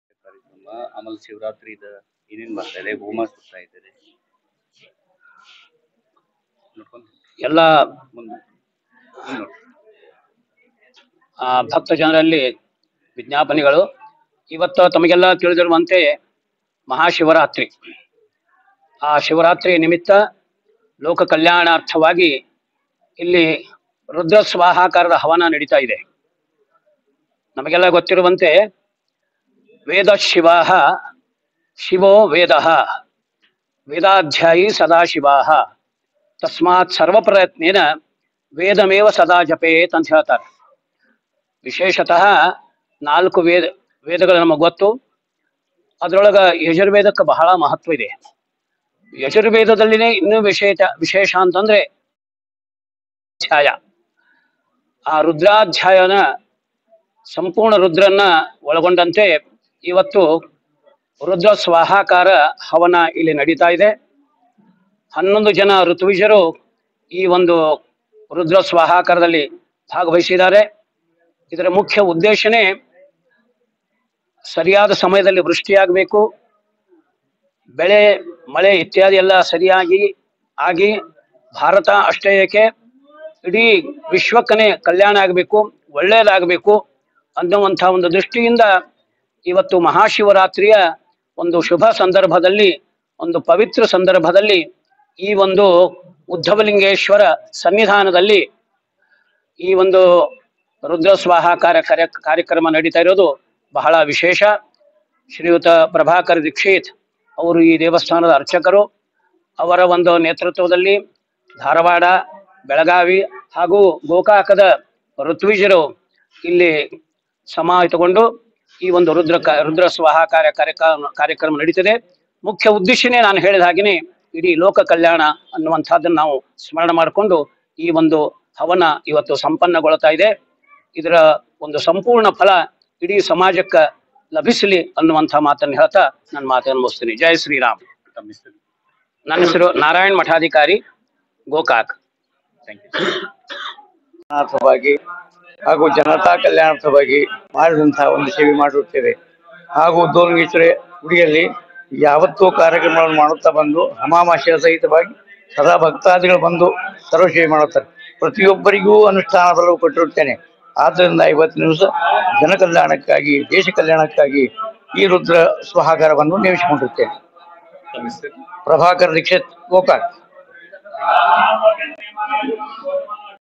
ಕರದಂಟು ನಗರಿ ಗೋಕಾಕದಲ್ಲಿ ಮಹಾ ಶಿವರಾತ್ರಿ ಸಂಭ್ರಮ.Gokak Shivratri
ಮಹಾಶಿವರಾತ್ರಿ ಅಂಗವಾಗಿ ಗೋಕಾಕ ಶ್ರೀ ಉದ್ಭವಲಿಂಗೇಶ್ವರ ದೇವಸ್ಥಾನ ಕಿಲ್ಲಾದಲ್ಲಿ ವಿಶೇಷ ಪೂಜೆ